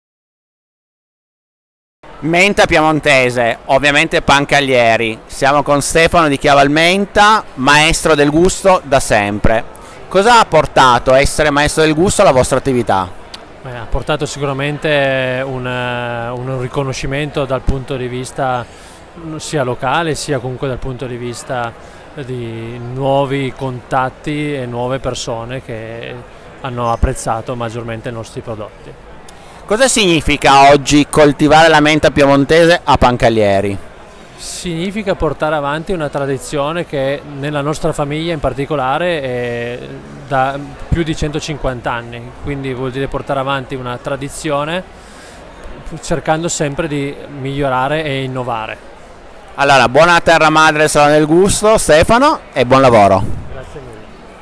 Stefano Ghirardi (Chialvamenta) – Terra Madre Salone del Gusto 2024
Stefano Ghirardi intervistato allo stand camcomtorino durante Terra Madre Salone del Gusto 2024